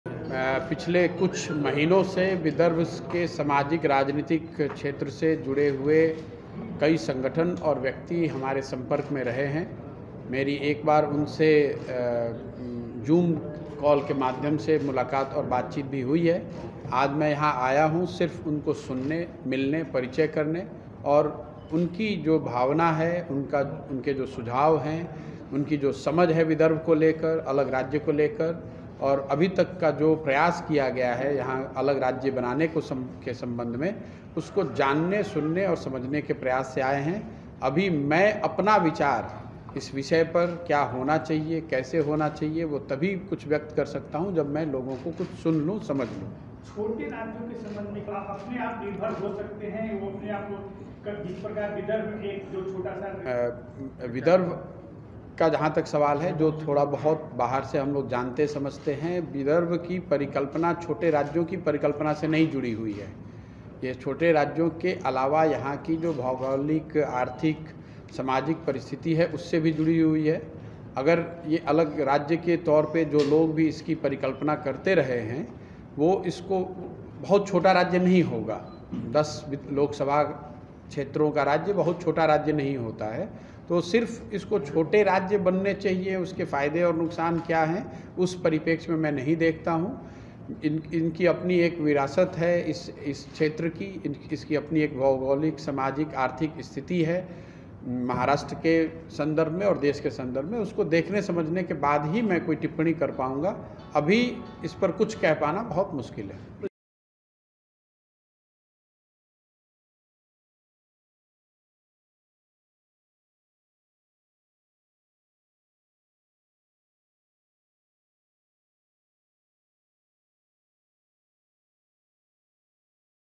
Prashant Kishor : 'वेगळ्या विदर्भाबद्दल लोकच बोलतील' | LetsUpp Marathi
स्वतंत्र विदर्भाच्या मागणीसाठी आज राजकीय रणनीतीकार प्रशांत किशोर आज नागपुरात दाखल झाले. त्यांनी विदर्भातील नेत्यांची भेट घेतली आहे. स्वतंत्र विदर्भाच्या मागणीसाठी बैठक पार पडली, स्वतंत्र विदर्भासाठी काम करत असेल्या सामाजिक राजकीय व्यक्तींसोबत मी आज जोडलो आहे. वेगळ्या विदर्भाबाबत विदर्भातील लोकच बोलणार असल्याचं ते म्हणाले आहेत.